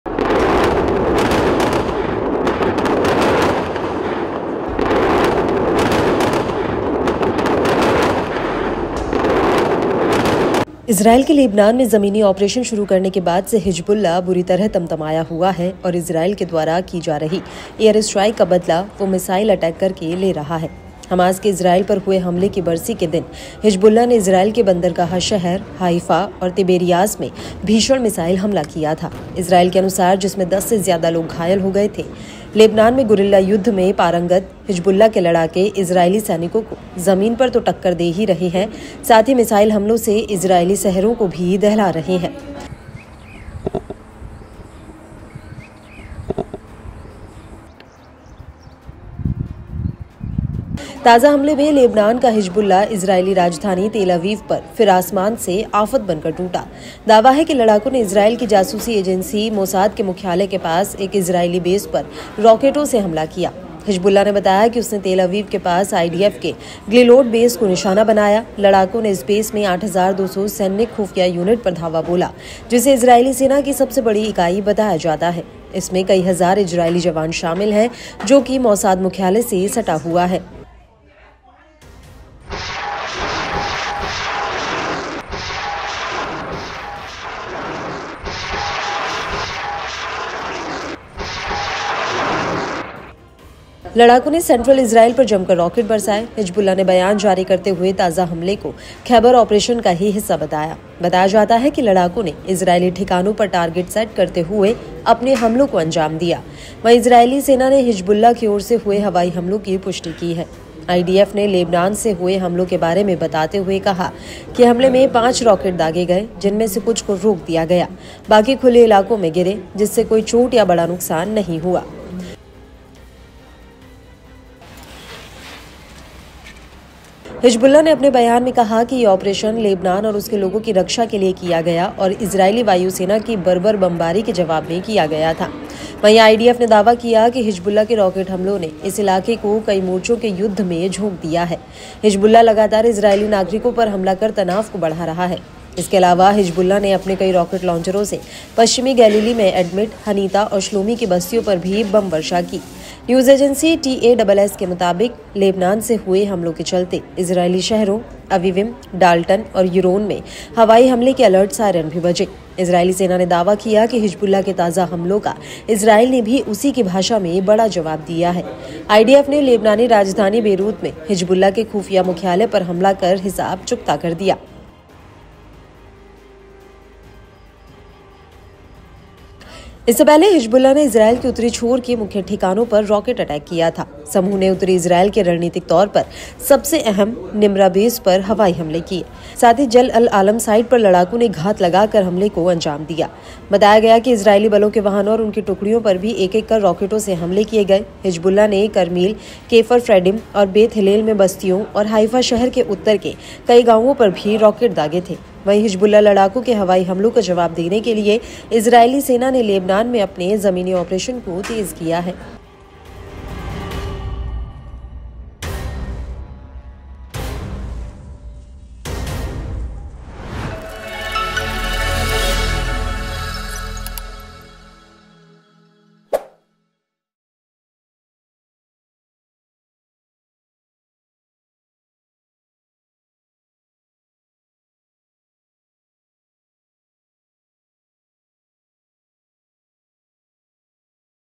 Hezbollah Attack Tel aviv: Israel के Mossad Headquarter को निशाना बनाकर फिर हमला | Lebanon | N18G
Hezbollah is very angry since Israel started ground operation in Lebanon. It is taking revenge of the air strikes being carried out by Israel by missile attacks. On the anniversary of Hamas' attack on Israel, Hezbollah carried out a fierce missile attack on Israel's port city Haifa and Tiberias, according to Israel, in which more than 10 people were injured. Hezbollah fighters, adept in guerrilla warfare in Lebanon, are not only fighting Israeli soldiers on the ground but are also terrorizing Israeli cities with missile attacks. In the latest attack, Lebanon's Hezbollah again attacked the Israeli capital Tel Aviv from the sky. It is claimed that the fighters attacked an Israeli base near the headquarters of Israel's intelligence agency Mossad with rockets. Hezbollah said that it targeted the IDF's Glilot base near Tel Aviv. The fighters attacked 8200 military intelligence units in this base. It is said to be the largest unit of the Israeli army. It comprises several thousand Israeli soldiers. It is adjacent to the Mossad headquarters.

इजरायल के लेबनान में जमीनी ऑपरेशन शुरू करने के बाद से हिजबुल्लाह बुरी तरह तमतमाया हुआ है. इजरायल के द्वारा की जा रही एयर स्ट्राइक का बदला वो मिसाइल अटैक करके ले रहा है. हमास के इज़रायल पर हुए हमले की बरसी के दिन हिजबुल्लाह ने इज़रायल के बंदरगाह शहर हाइफा और तिबेरियास में भीषण मिसाइल हमला किया था इज़रायल के अनुसार जिसमें 10 से ज्यादा लोग घायल हो गए थे. लेबनान में गुरिल्ला युद्ध में पारंगत हिजबुल्लाह के लड़ाके इज़रायली सैनिकों को ज़मीन पर तो टक्कर दे ही रहे हैं साथ ही मिसाइल हमलों से इज़रायली सेहेरों को भी दहला रहे हैं. ताज़ा हमले में लेबनान का हिजबुल्लाह इजरायली राजधानी तेल अवीव पर फिर आसमान से आफत बनकर टूटा. दावा है कि लड़ाकों ने इजरायल की जासूसी एजेंसी मोसाद के मुख्यालय के पास एक इजरायली बेस पर रॉकेटों से हमला किया. हिजबुल्लाह ने बताया कि उसने तेल अवीव के पास आईडीएफ के ग्लिलोट बेस को निशाना बनाया. लड़ाकों ने इस बेस में 8200 सैन्य खुफिया यूनिट पर धावा बोला. जिसे इजरायली सेना की सबसे बड़ी इकाई बताया जाता है. इसमें कई हज़ार इजरायली जवान शामिल हैं. जोकि मोसाद मुख्यालय से सटा हुआ है.